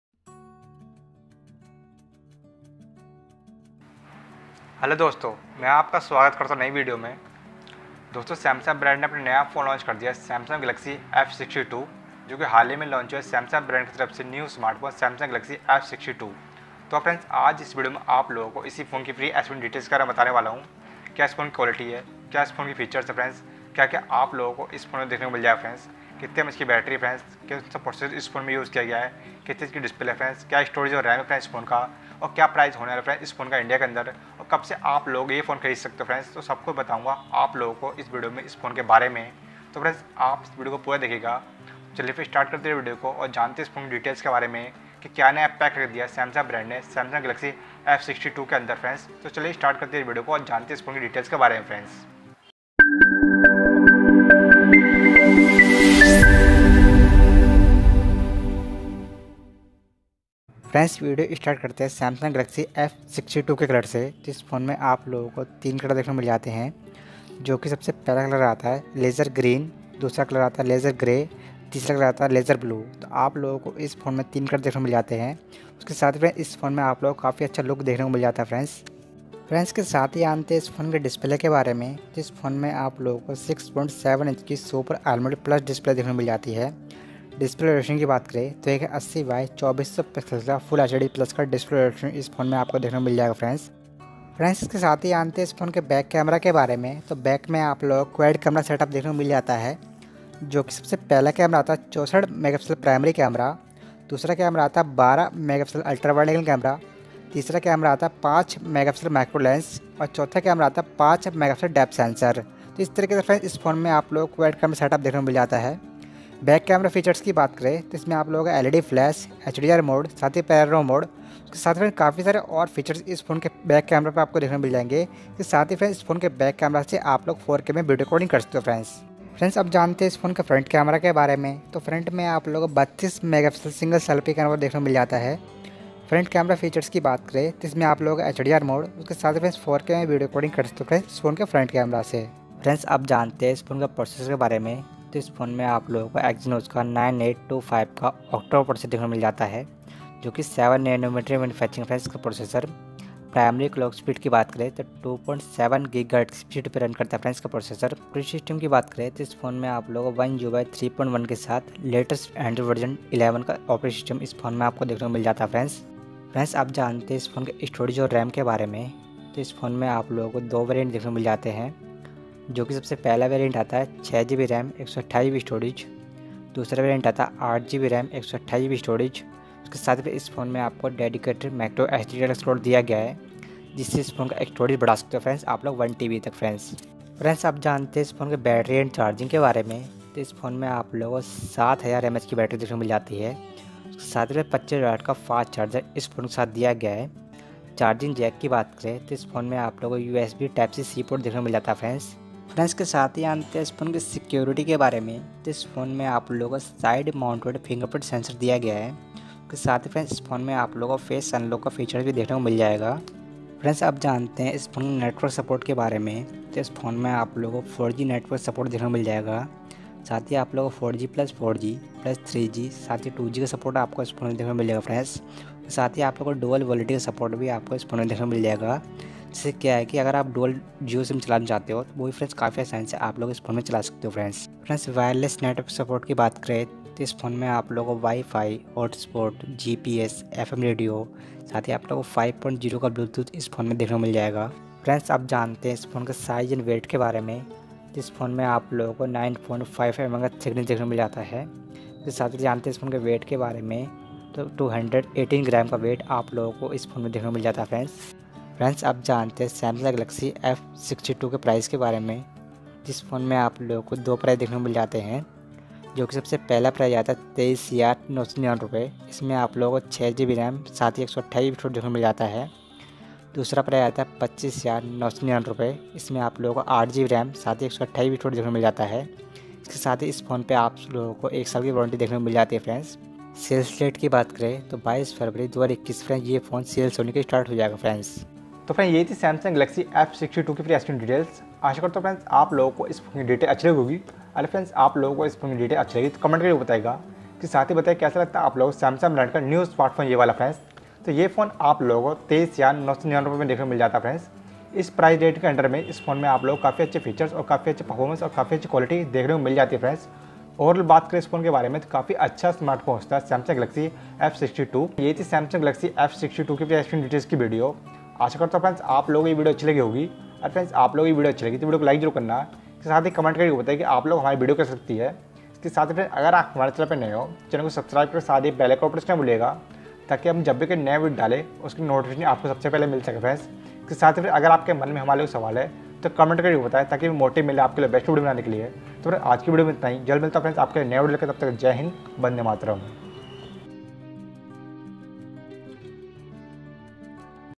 Samsung Galaxy F62|Galaxy F62 Unboxing & full Specification and Parfums and Features,First Interface
By. monu Kumar

Selle Mobile

( Galaxy F62 Full specs )

( Colors )
1. Laser Green
2. Laser Grey
3. Laser Blue
Good luck

( Display )
6.7 inches Super AMOLED Plus Display
Display Resolution- 1080×2400 Pixels FHD+ Display Resolution

( Back Camera ) Quad Camera
1. 64MP Primary Camera
2. 12MP Ultra Wide Angle
3. 5MP Macro Lens
4. 5MP Depth Sensor
Features- LED Flash, HDR Mode, Panorama Mode, & Other features, with 4K video Recording

( Front Camera )
1. 32MP Single Selfie camera
Features- HDR Mode, With 4K video Recording

( Processor )
Exynos 9825 Octa Core Processor ( 7nm )
Primary Clock Speed- 2.7GHz Speed Run
Operating system_ OneUI 3.1 With Latest Android v11 (OS)

( Storage and RAM )
1. 6GB Ram/128GB Storage
2. 8GB Ram/128GB Storage
With Dedicated MicroSD Card Slot Storage Up to 1TB

( Battery and Charger )
7000mAh Battery with 25W Fast Charger
Charging Jack- USB Type-C Port

( Security )
Side Mounted Fingerprint Sensors With Face Unlock Features

( Network support )
4G network support
4G+4G,3G,2G network support
Wireless Network- Wifi, Hotspot, GPS, FM Radio, v5.0 Bluetooth

Thickness - 9.5mm
Weight - 218g

( Mobile Price )
1. Rs 23,999 6GB/128GB
2. Rs 25,999 8GB/128GB
1 Year warranty Sells On 22 February 2021

( To follow us हमें फोलो करने के लिए )
M.K Tech Knowledge channel

( Content cowords )
Galaxy F62, Galaxy F62 Specification, F62,
Best Smartphones,
Mobile
Mobile phone
Phone
Smartphones